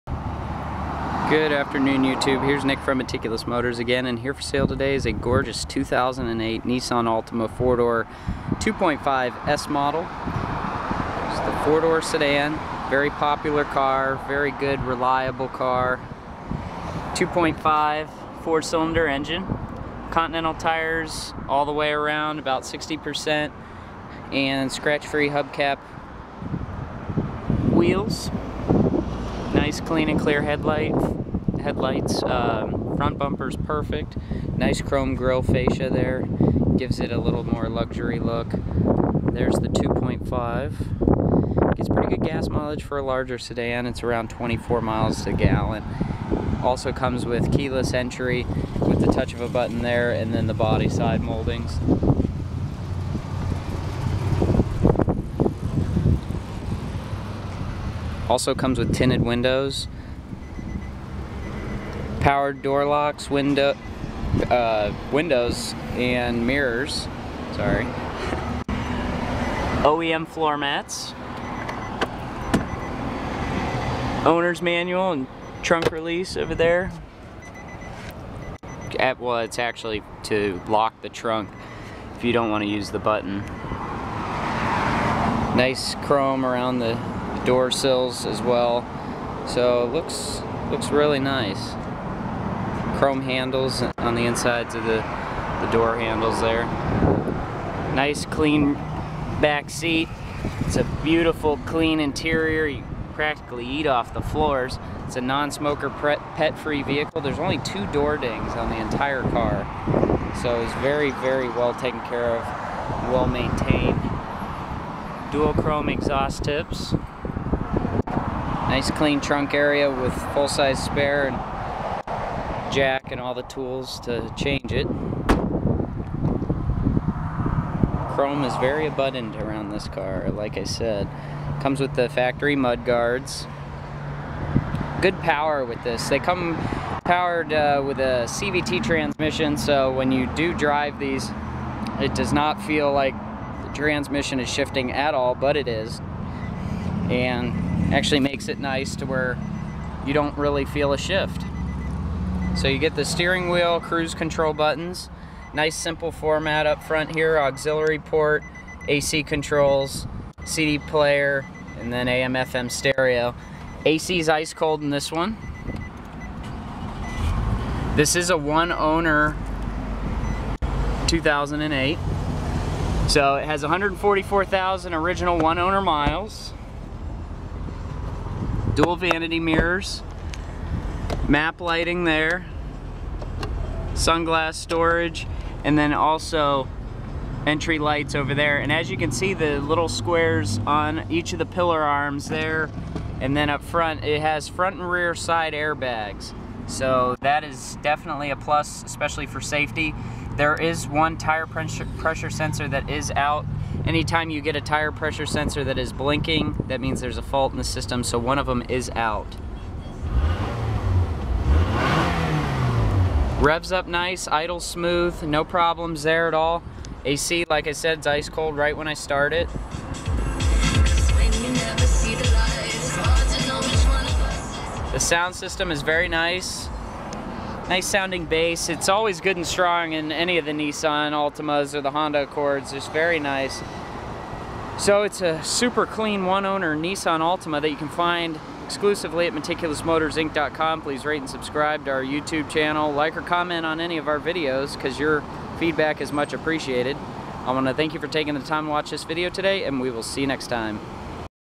SOLD 2008 Nissan Altima 2.5 S One Owner Meticulous Motors Inc Florida For Sale
Up for sale is a one owner one minor accident AutoCheck Certified 2008 Nissan Altima 2.5 S. This Nissan comes with power windows/locks/mirrors, AUX/iPod input, ice cold A/C, and more! Come by and drive away in this economical sedan today!